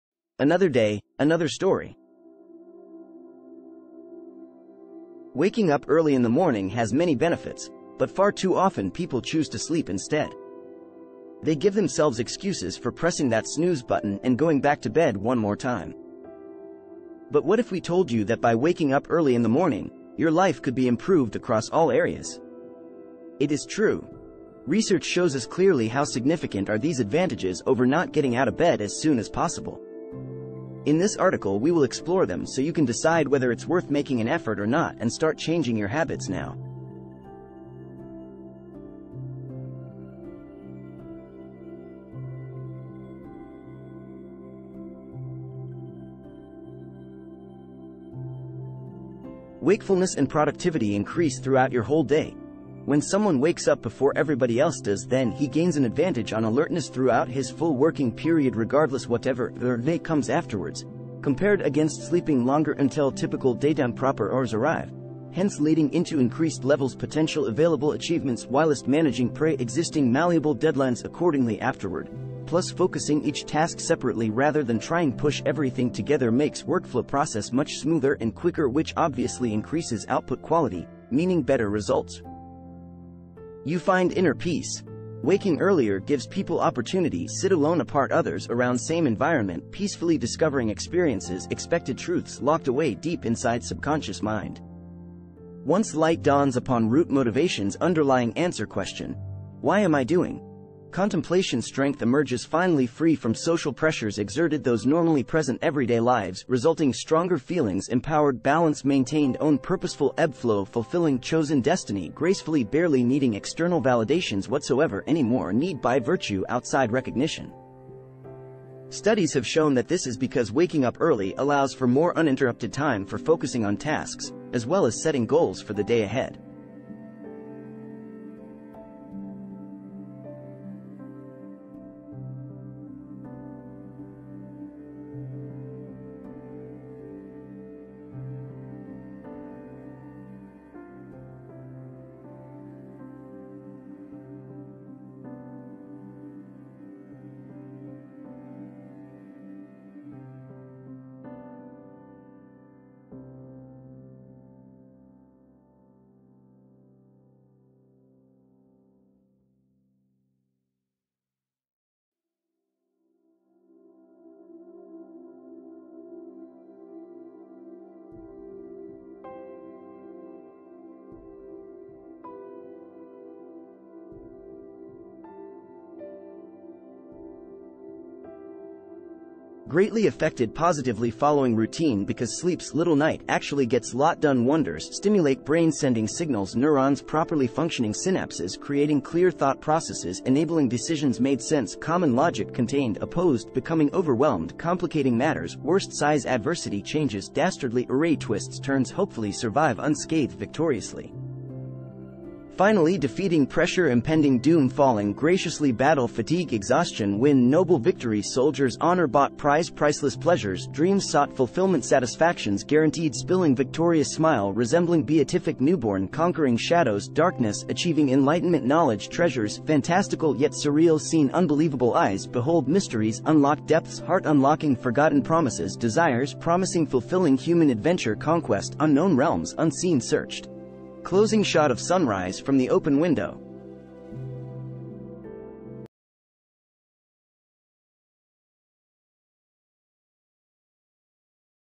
The Benefits of Waking Up Early In The Morning | wakeup early
This video will provide a comprehensive guide on how to wake up early in the morning and make it a habit. You'll learn tips and tricks to create an effective morning routine, as well as advice on getting motivated each day. Additionally, you'll receive recommendations for helpful habits that can help increase energy levels throughout the day."wake up early", "morning motivation", "effective routines" and "healthy habits". @Well-beingTipsandTricks @aupmanyu